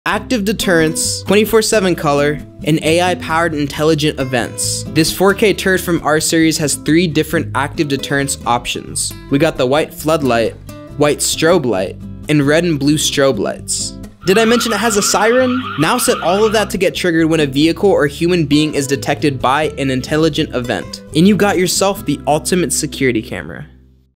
Unbelievable Home Defense - 4K Turret with 3 Deterrent Lights and a *Siren*!!
This turret is JAM-PACKED with features!! 🤯 Check out the full video up on the channel.